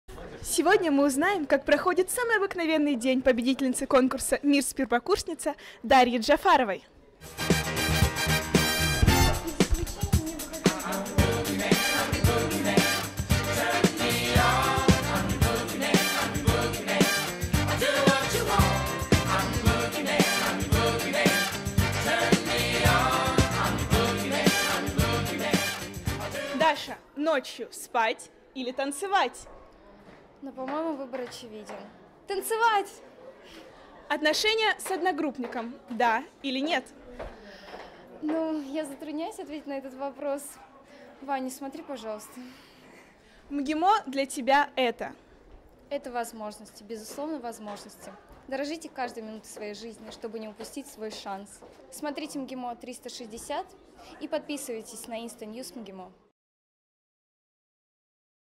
Обычный день студентки МГИМО
Все мы погружены в суету будней, но порой некоторые из нас радуют себя и других. Вот и победительница конкурса Мисс Первокурсница по версии Instanewsmgimo порадовала нас.